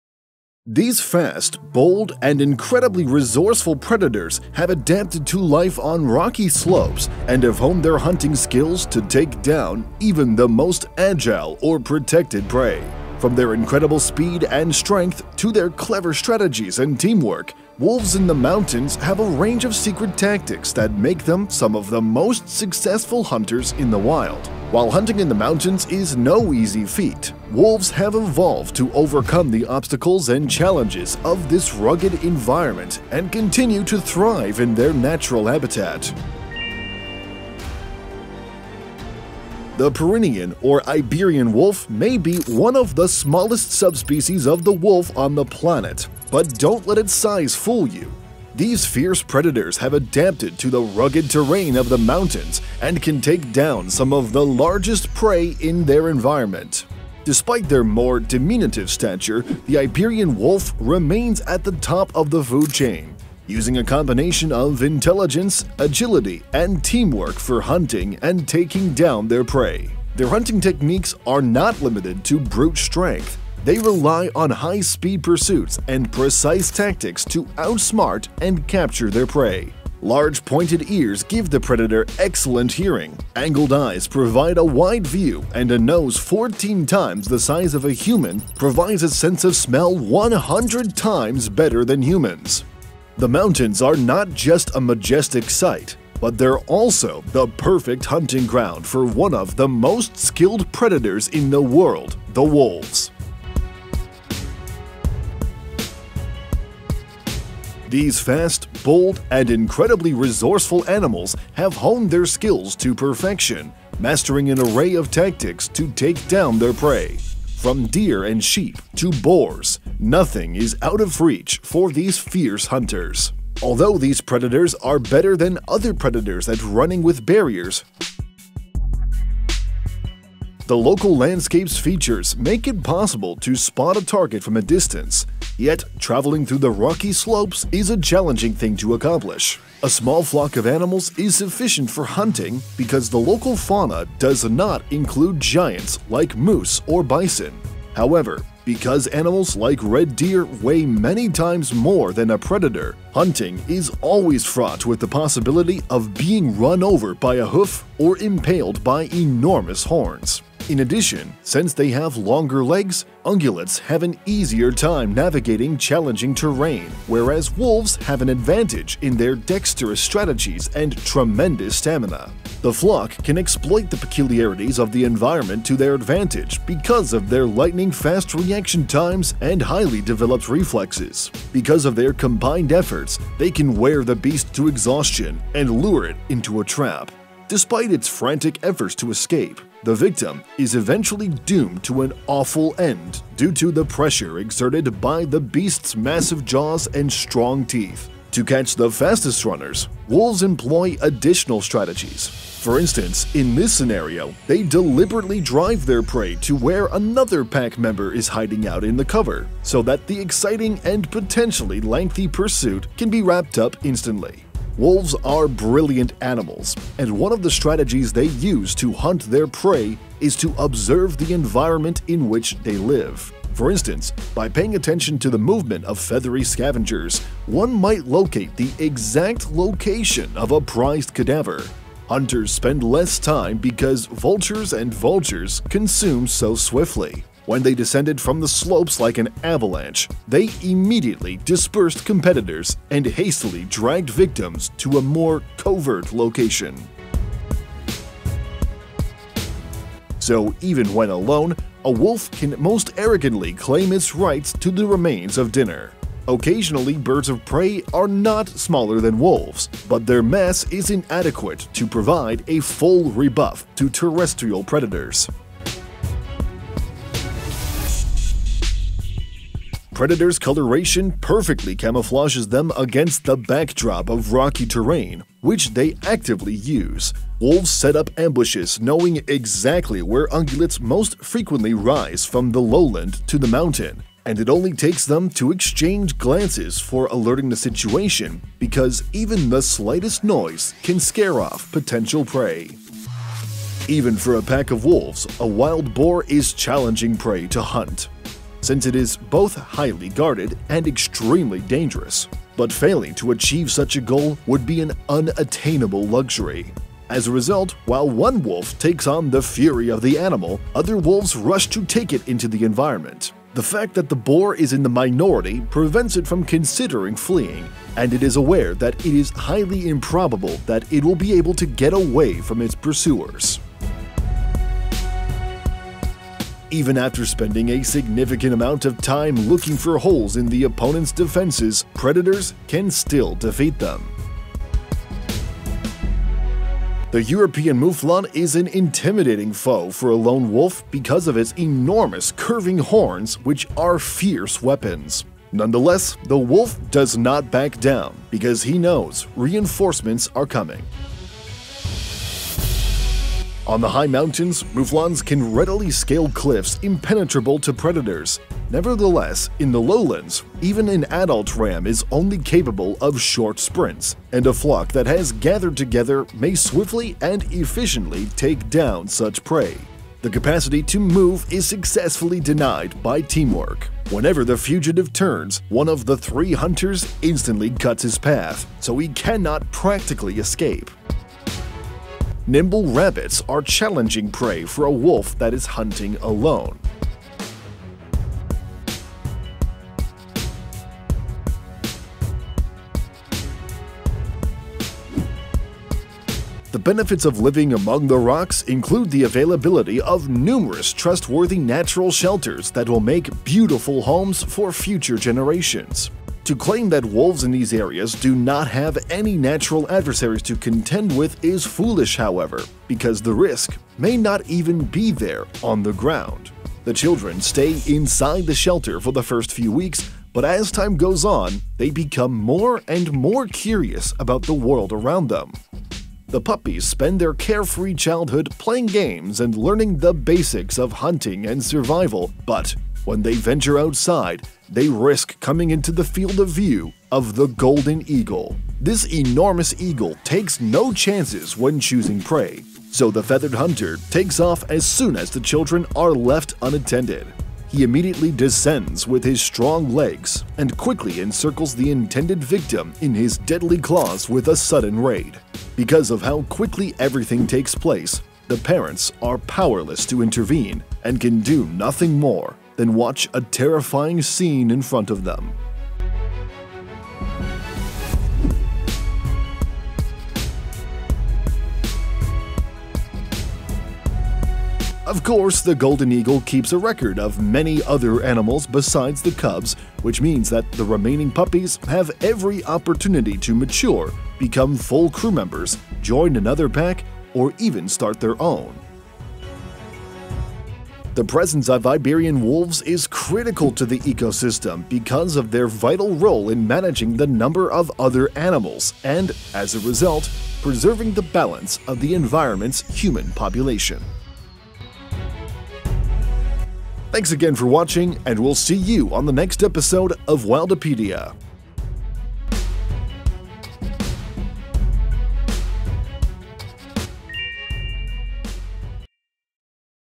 WOLF PACK: This is How Wolves Hunt in the Mountains
These fast, bold, and incredibly resourceful predators have adapted to life on rocky slopes and have honed their hunting skills to take down even the most agile or protected prey.
From their incredible speed and strength to their clever strategies and teamwork, wolves in the mountains have a range of secret tactics that make them some of the most successful hunters in the wild.
While hunting in the mountains is no easy feat, wolves have evolved to overcome the obstacles and challenges of this rugged environment and continue to thrive in their natural habitat.

The Pyrenean or Iberian wolf may be one of the smallest subspecies of the wolf on the planet, but don't let its size fool you. These fierce predators have adapted to the rugged terrain of the mountains and can take down some of the largest prey in their environment.
Despite their more diminutive stature, the Iberian wolf remains at the top of the food chain, using a combination of intelligence, agility, and teamwork for hunting and taking down their prey.
Their hunting techniques are not limited to brute strength; they rely on high-speed pursuits and precise tactics to outsmart and capture their prey.